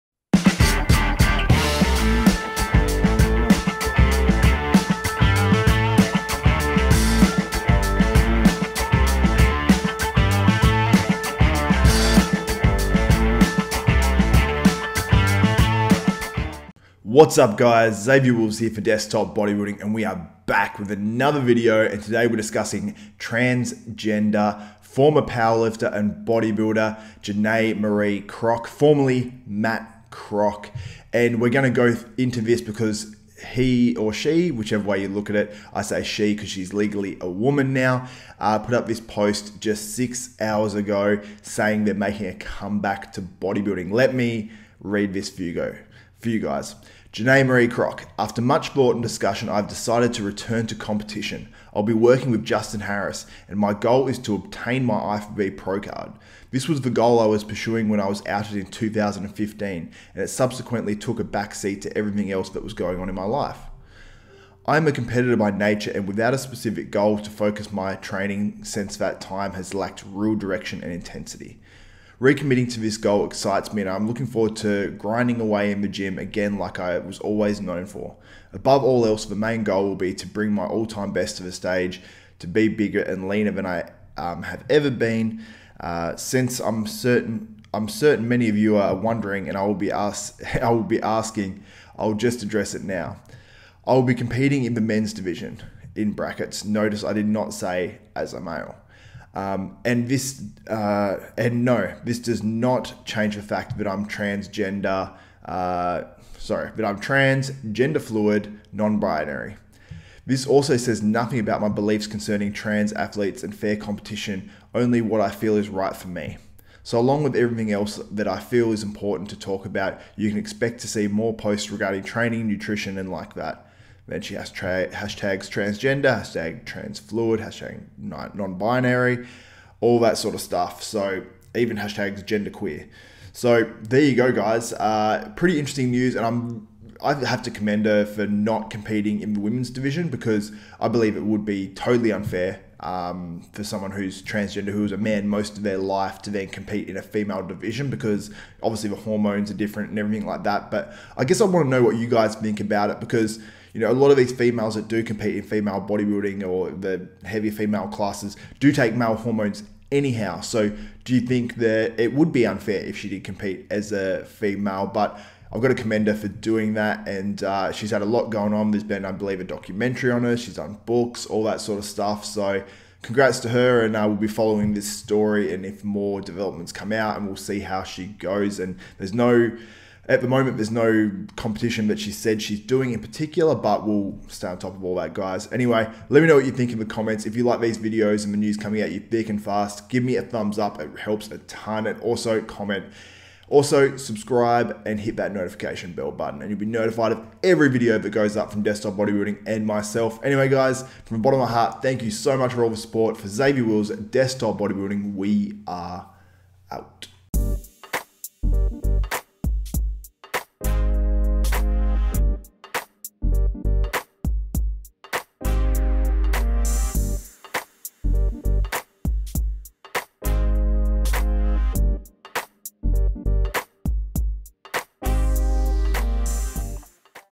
Trangender Bodybuilder Janae Kroc to compete in Bodybuilding again (formerly Matt Kroc)
Transgender former Powerlifter/Bodybuilder Janae Kroc (formerly Matt Kroc) has decided to compete in bodybuilding again, but this will be the first time competing since being out as transgender. Do you think Janae Kroc has any chance of winning her IFBB Pro Card?